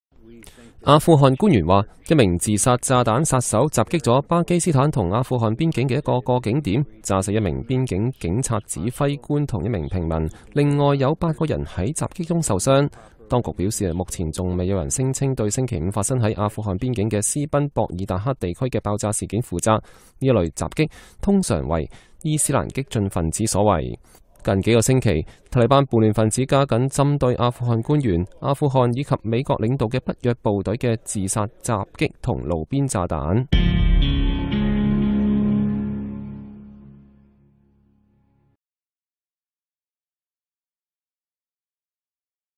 巴阿邊界自殺炸彈造成多人傷亡
阿富汗官員說，一名自殺炸彈手襲擊了巴基斯坦和阿富汗邊境的一個過境點，炸死一名邊境警察指揮官和一名平民，另有8人在襲擊中受傷。當局表示，目前還沒有人聲稱對星期五發生在阿富汗邊境的斯賓-博爾達克地區的爆炸事件負責。此類襲擊通常為伊斯蘭激進份子所為。